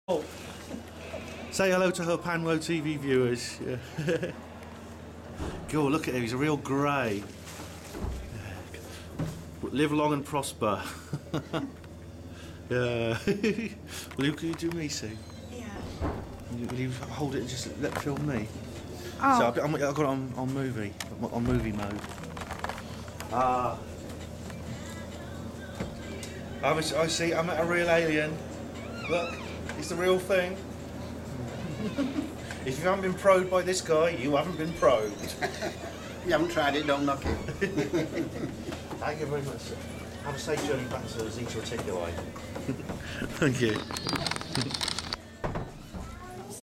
UK Probe Conference 2010 Part 5- An Alien in our Midst!
An alien suddenly appeared in the conference auditorium and tried to make contact with some of us. It's just the kind of thing Dex would have loved to have seen... but for some reason he'd gone off somewhere and couldn't be found.